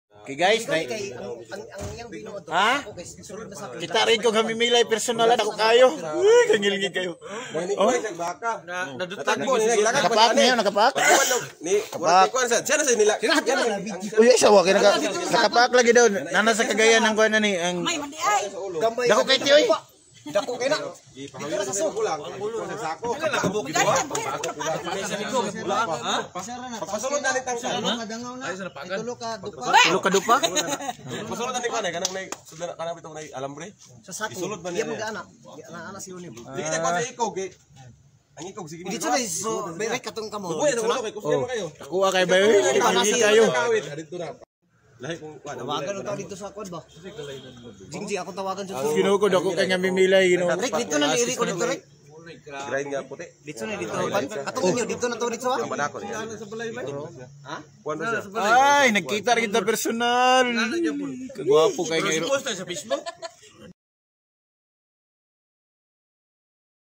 Amimilay or King Cobra!
Ang Amimilay or kilala sa tawag na King Cobra ay matatagpuan dito sa aming lugar sa Malaybalay City Bukidnon. Marami daw nito sa Barangay Mabuhay.